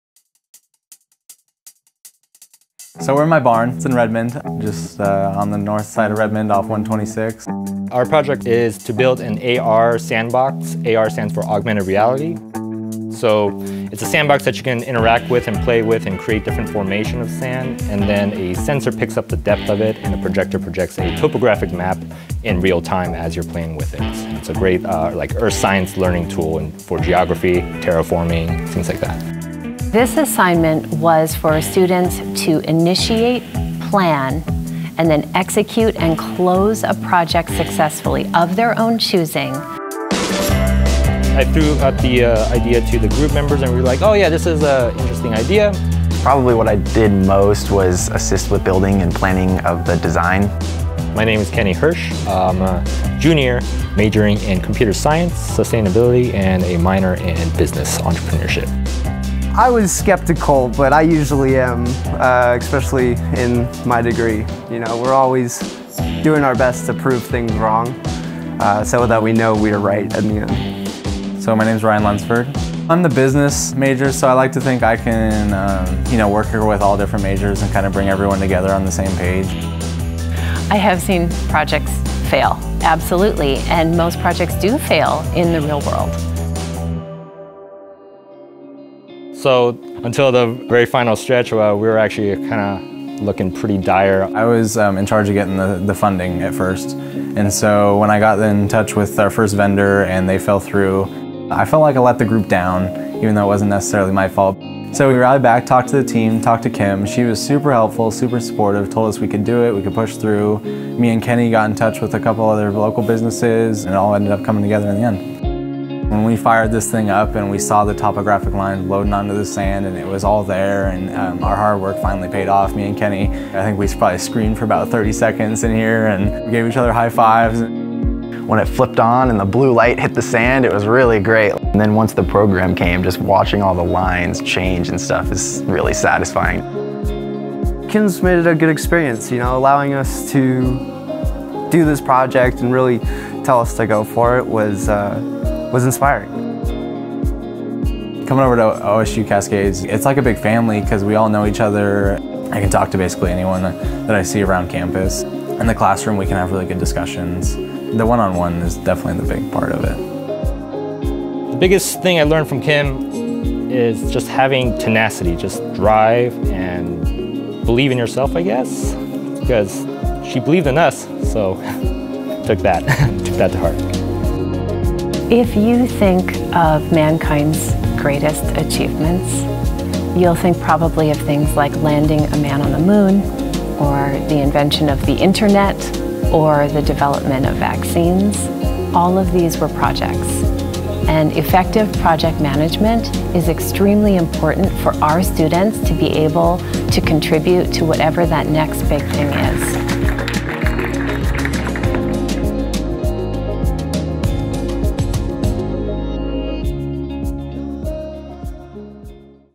Business Management - Building an AR Sandbox
In Management 364, a business administration major, a computer science major, and two energy systems engineering majors came together to plan and execute a group project: building an augmented reality sandbox that projects a topographic map in real time.